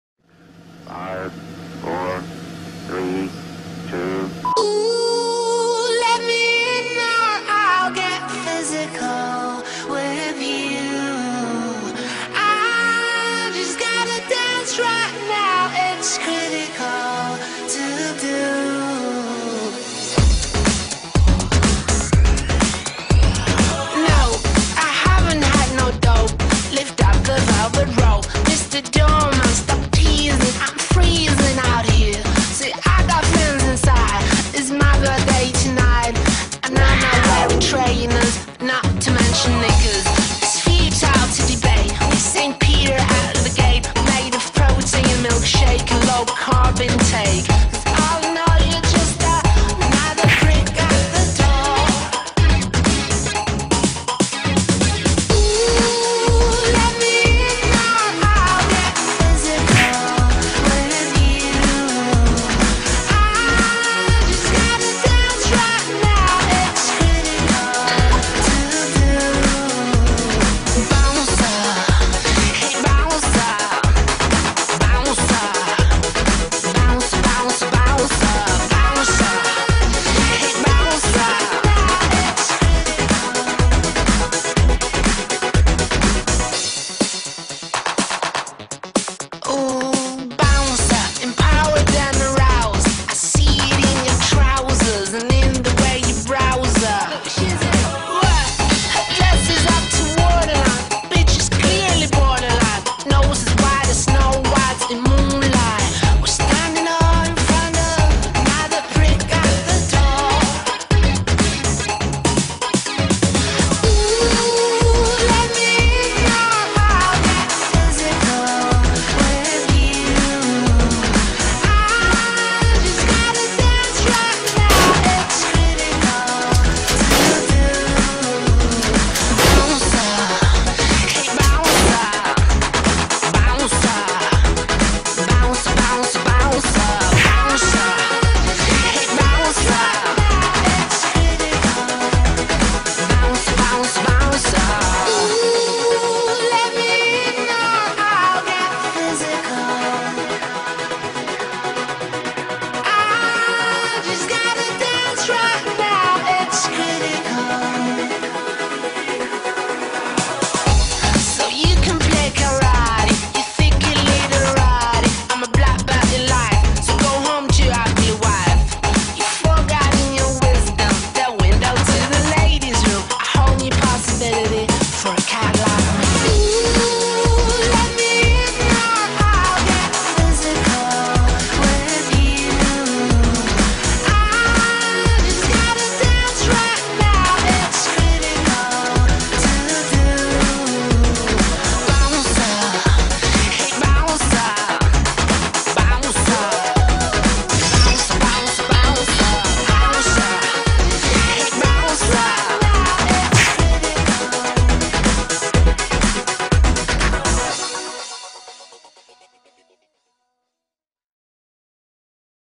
Badr HARI vs Gökhan SAKI - 2012 - HD - "Bouncer"
The Goldenboy" Hari's last fight is against "The Rebel" Saki -  January / 28 / 2012